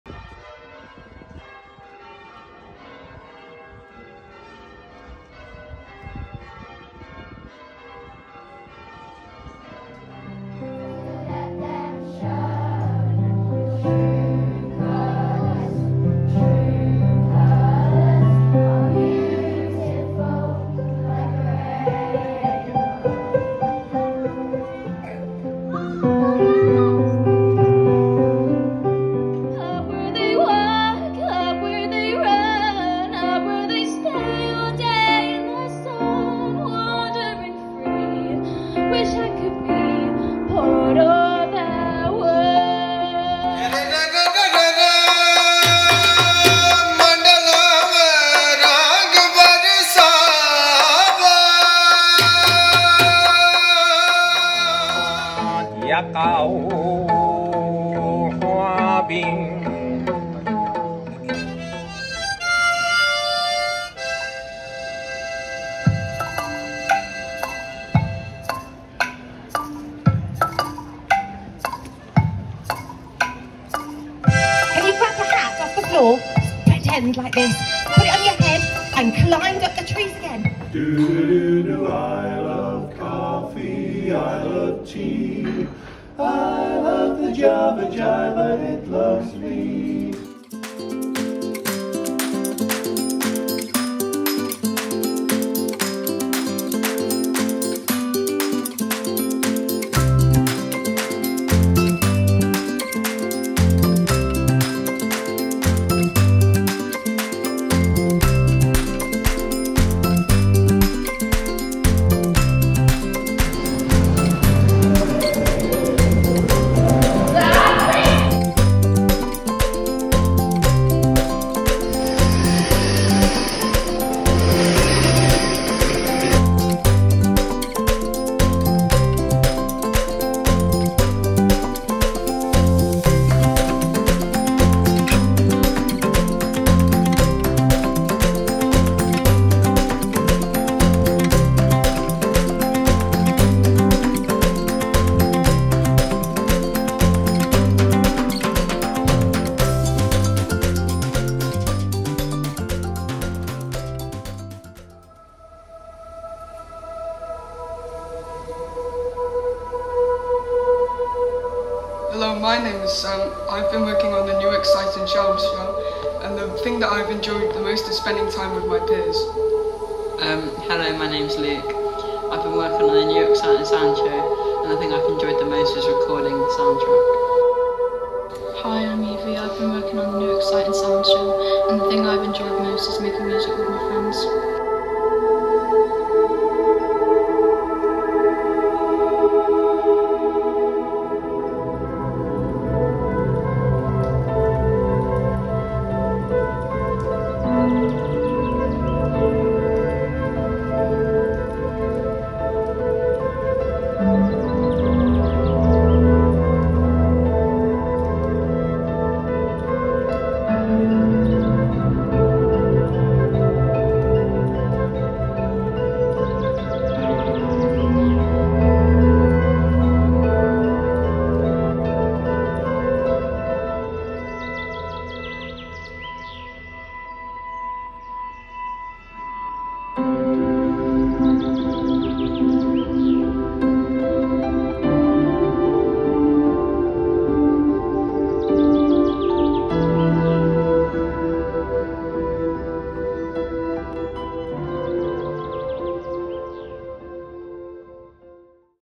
Newark Creates 2023
This video highlights our projects during the 2023 High Street Heritage Action Zone Cultural Programme.